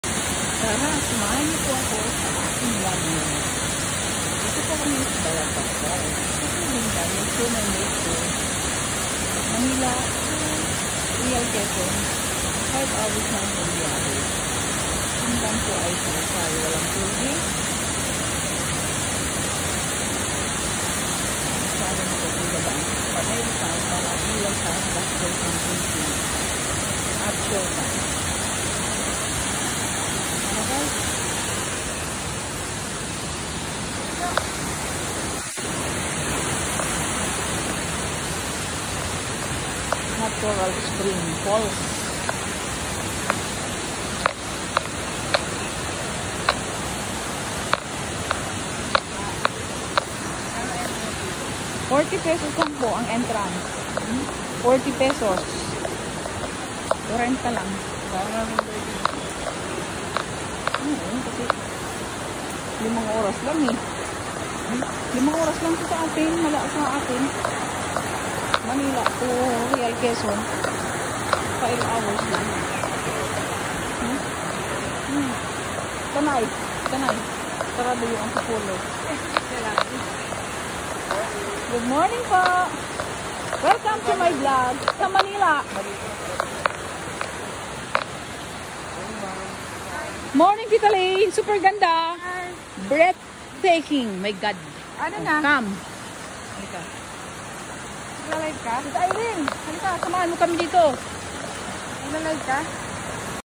Balagbag Falls /Real Quezon
Super ganda 5 hrs from Manila to Real Quezon @BalagbagFalls